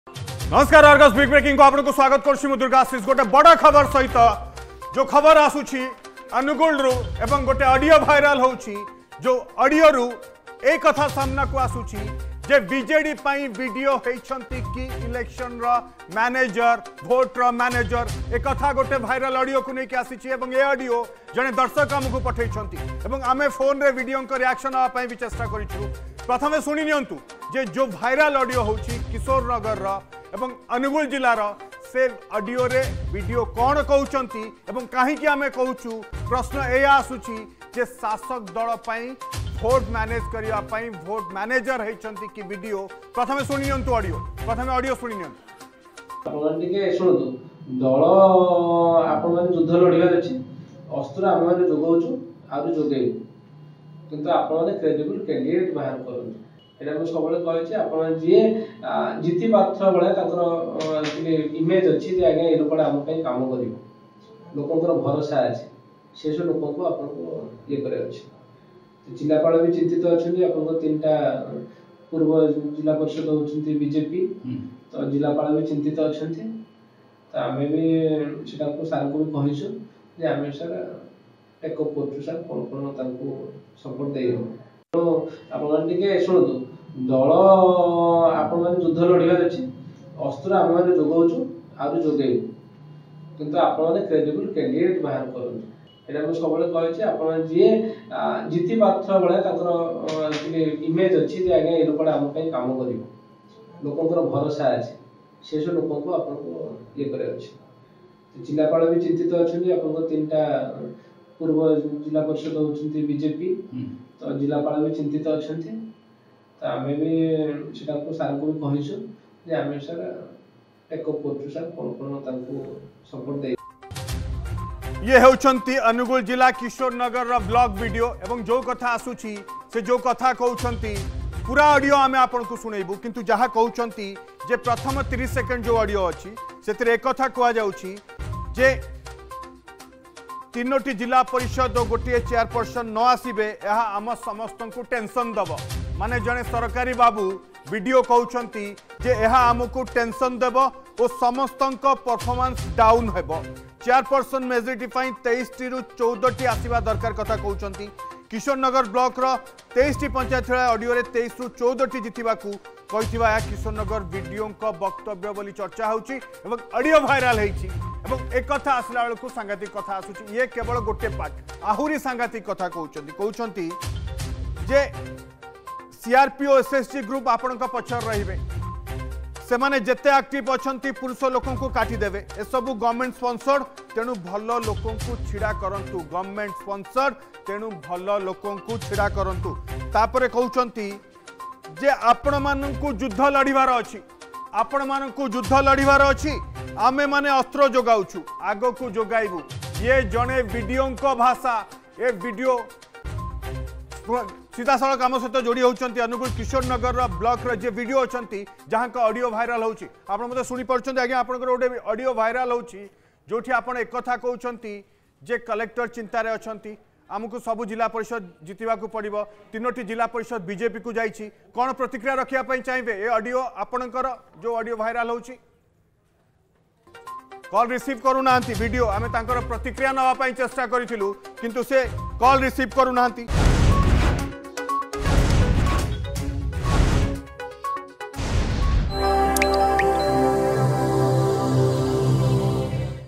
Angul: BDO Involved in Managing Votes for BJD in the Panchayat Election
Angul district Kishornagar block BDO in dispute. BDO's controversial audio is viral. BDO Involved in Managing Votes for BJD in the Panchayat Election.